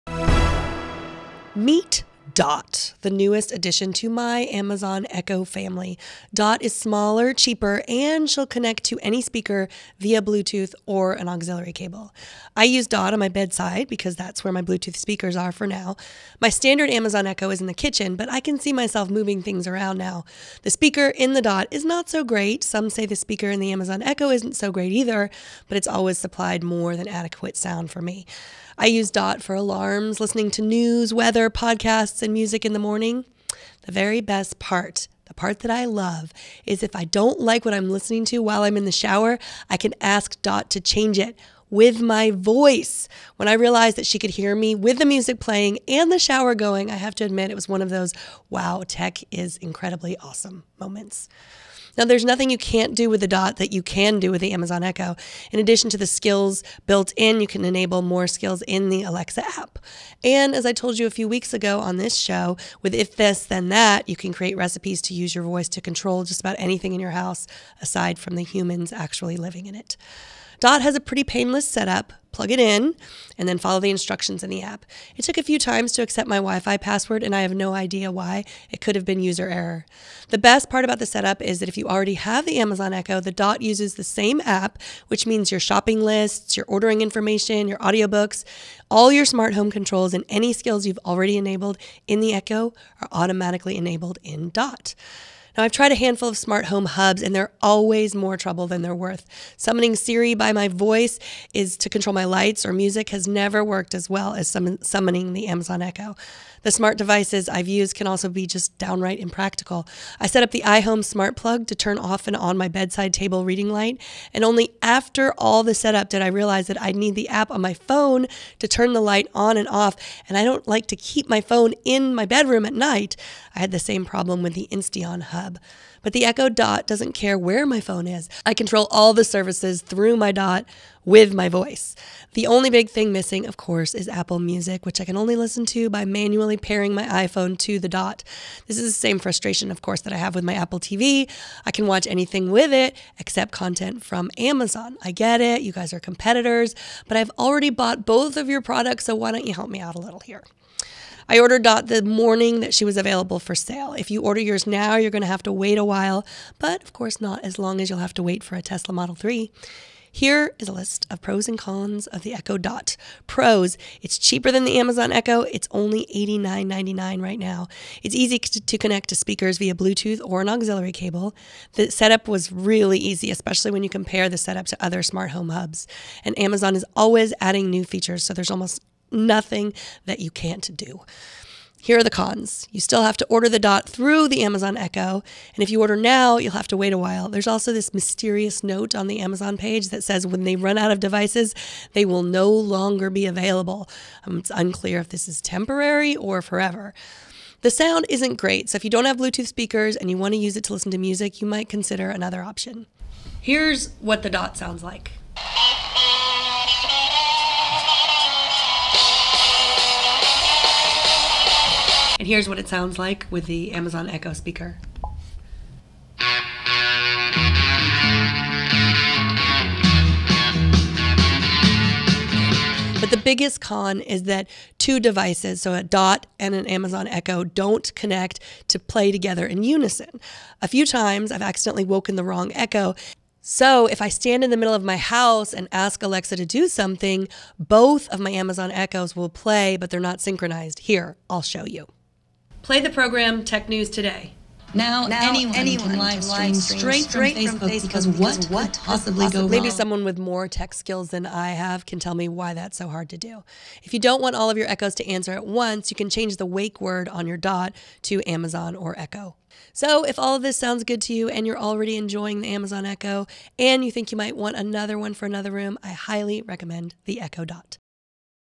How to Use the Amazon Dot
The Amazon Dot has arrived and Megan Morrone show you how it works, some best practices, and what still needs to be worked out with this new personal digital assistant.